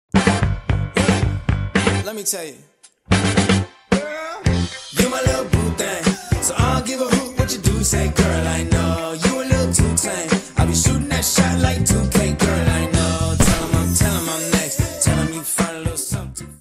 Before and After Rhinoplasty, Breast Augmentation in Korea 📩
Plastic surgery made her more confident! She has done Rhinoplasty, Breast Augmentation, Fat Graft and Thread Lift  🥰

📢 ‼️ Promotion for deposit by August 2023!! Up to 35% surgery discount!